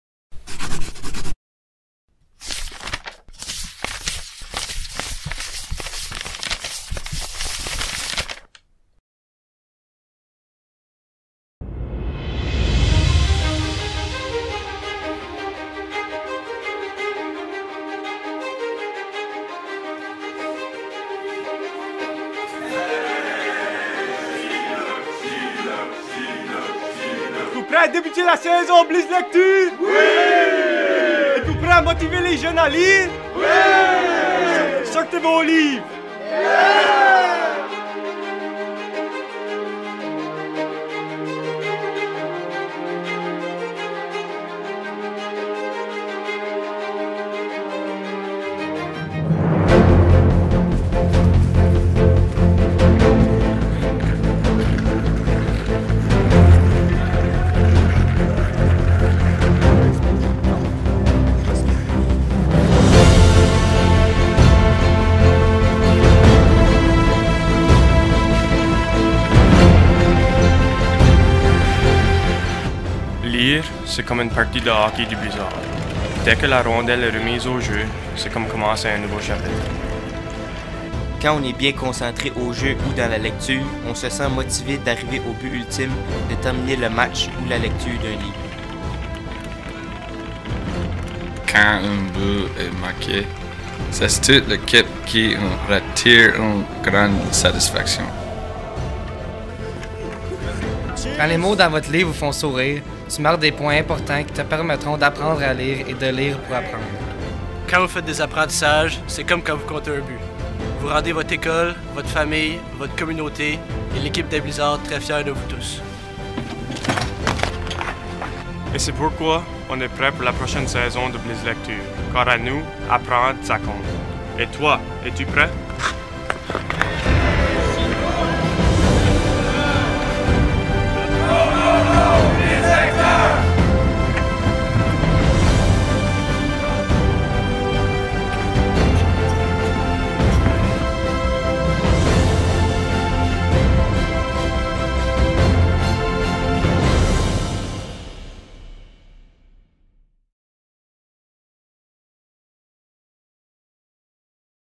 BLIZZLECTURE 2022 (capsule vidéo de lancement)
Le District scolaire francophone du Nord-Ouest (DSFNO) continue son alliance avec le Blizzard d'Edmundston (une équipe de la Ligue de hockey junior A des Maritimes) et orchestre une nouvelle saison de Blizzlecture malgré la pandémie.

Le Blizzlecture est un volet d’un programme de littératie communautaire créé par le DSFNO. Ce programme vise principalement à motiver des jeunes garçons de la 3e à la 5e année à lire davantage pour le plaisir. Parce que lire, ça compte!

Les participants — les Blizzlecteurs — doivent s’engager à lire 75 minutes par semaine à la maison, avec l’appui de leurs parents. Pour les encourager à remplir leur contrat, le DSFNO a acheté et mis à leur disposition une variété de livres correspondants aux intérêts des garçons de leur âge. Les Blizzlecteurs ont autant accès à des livres en papier qu’à une bibliothèque virtuelle.

Le DSFNO a également créé des cartes de hockey à l’effigie des joueurs du Blizzard, à collectionner au fil des semaines. Chaque carte contient un message, un truc ou un conseil en lien avec la lecture, de la part du hockeyeur qui y est en vedette.

Les joueurs et les entraineurs du Blizzard ont aussi participé à la création de capsules vidéos pour encourager les Blizzlecteurs.

Pour la saison 2022, le programme de littératie communautaire du DSFNO compte deux volets, c’est-à-dire deux équipes de jeunes lecteurs. En plus des Blizzlecteurs, les Raplecteurs sont également de la partie! Ceux-ci sont aussi parrainés par une équipe Ligue de hockey junior A des Maritimes, soit les Rapides de Grand-Sault.